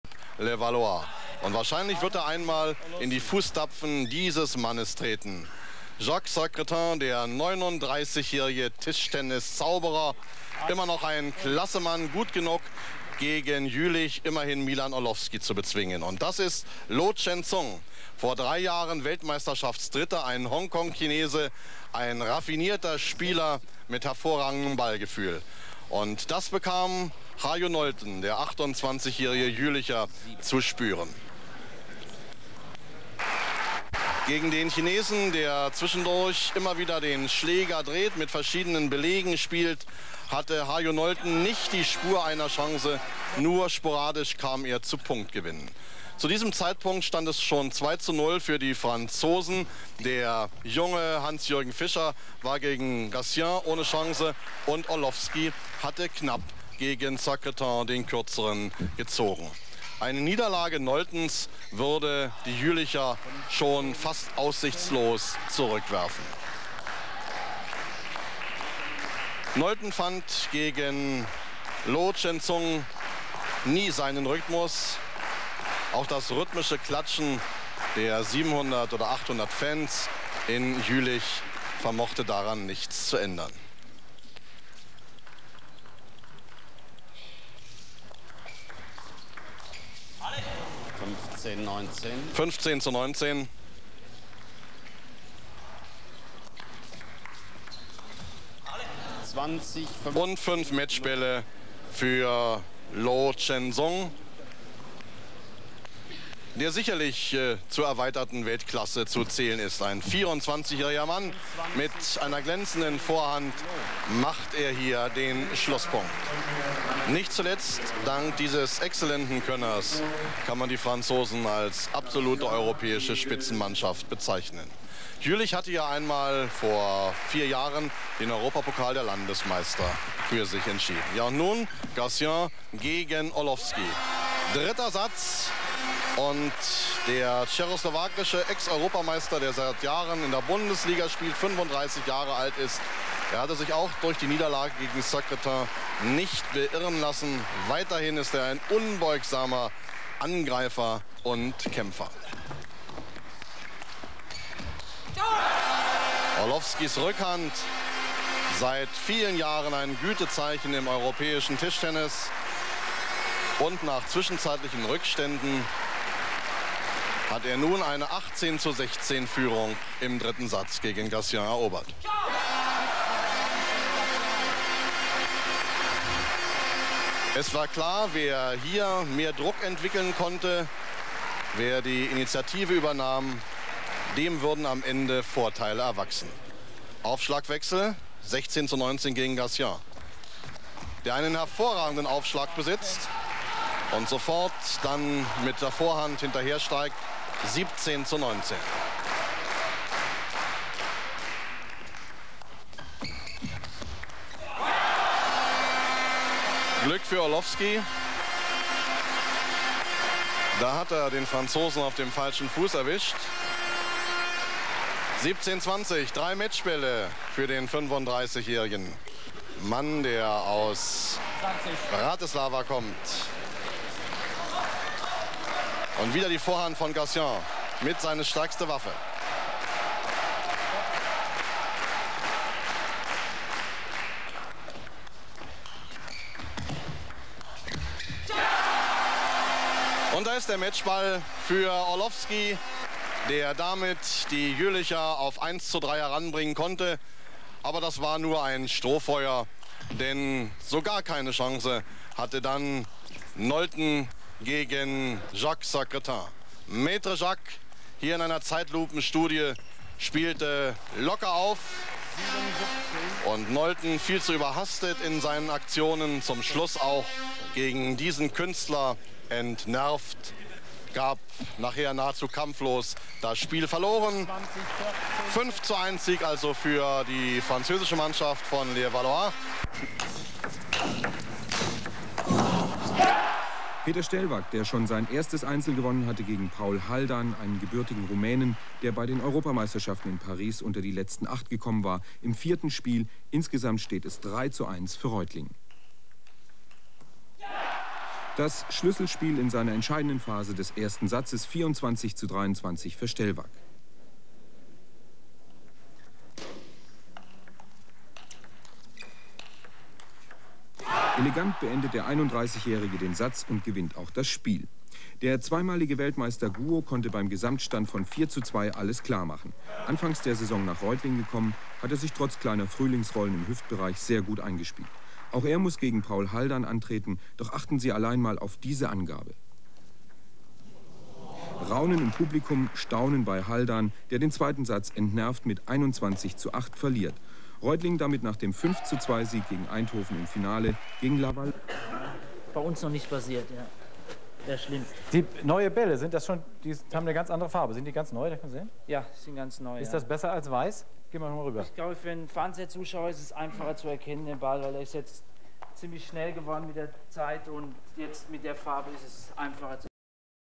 J. Secretin Defense -M.Orlowski (European Champion)schlägt J.P. Gatien Worldchampion 1993
TTC Juelich - Levallois) J.Secretin (39) beating Orlowski --HAJO Nolten (German Topplayer)
Milan Orlowski (European Champion) (35) beating Jean -Philipp Gatien
(Worldchampion 1993 vs. Saive)
Secretin - Nolten
Reutlingen-Eindhoven Peter Stellwaag GUO (Master of Serve 2x Worldchampion